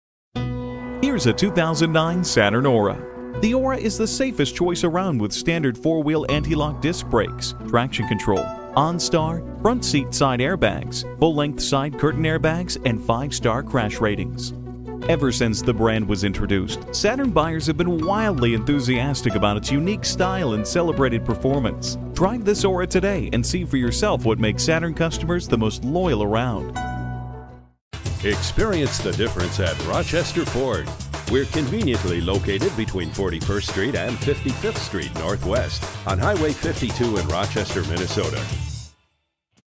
2009 Saturn Aura Rochester Minneapolis, MN AM12367 - SOLD
Year: 2009
Make: Saturn
Model: Aura
Trim: 4dr Sdn V6 XR FWD 3.6
Engine: 3.6 L V6 Cyl.
Transmission: 6-Speed A/T
Color: POLAR WHITE
Interior: Black
Mileage: 65332
Stock #: AM12367
If you are looking for a vehicle with all the options, then you found it. This powerful V6 sedan comes equipped with leather, heated seats, sunroof and more. It went through our shop and needing nothing more than an oil change and a mild detail. The previous guest took great care of it. Clean car! We look forward to earning your business.

Address:
4900 Hwy 52 North